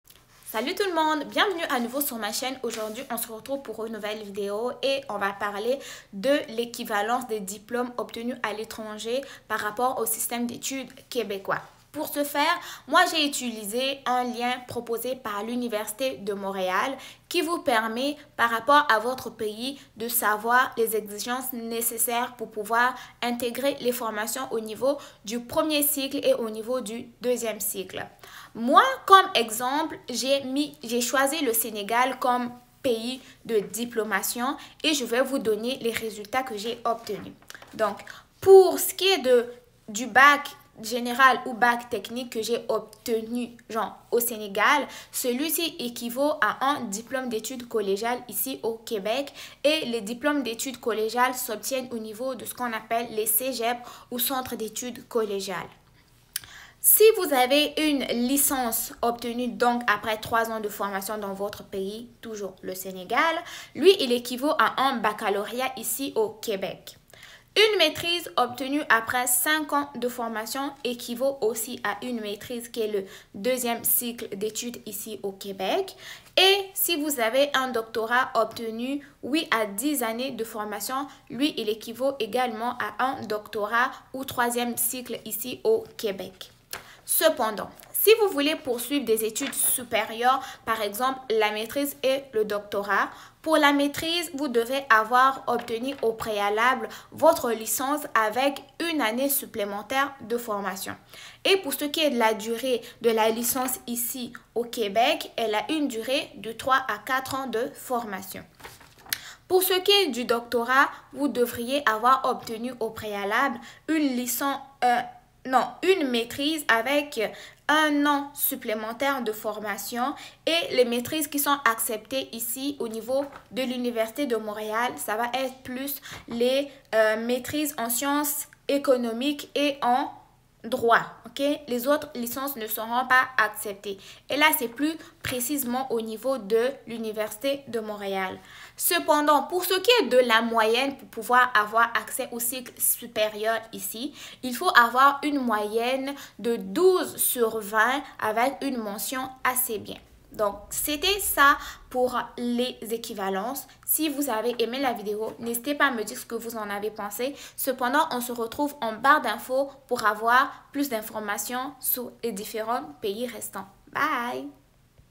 Équivalence des Diplômes Étrangers avec le système Canadien (Québec)
Bonjour, dans cette vidéo je te propose la correspondance de ton diplôme étranger avec son équivalence au Québec

Voici le lien qui peut t’aider à trouver ton équivalence par rapport à ton pays. Clique sur le lien pour connaître ton pays